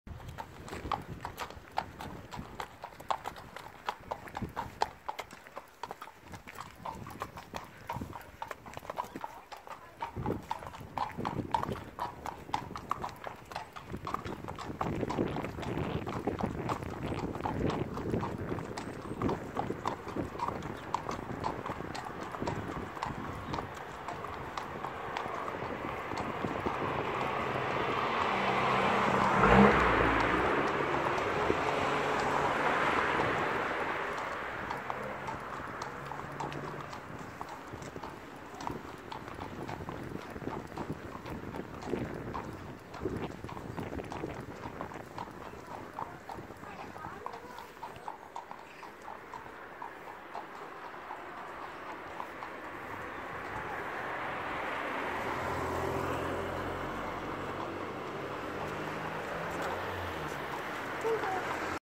Magic - Welsh Cob Gelding: Out on the Road
Magic. 14.3hh 6 yo chunky unregistered Welsh cob gelding. Great project for someone to bring back into full work as he's not done much since last year, but was happy for us to get straight on and ride him again. Magic has a sweet, loving, calm temperament and is ready to go on into any sphere with work. Good home sought. Passport, microchipped and vaccinated up to date. £950 ono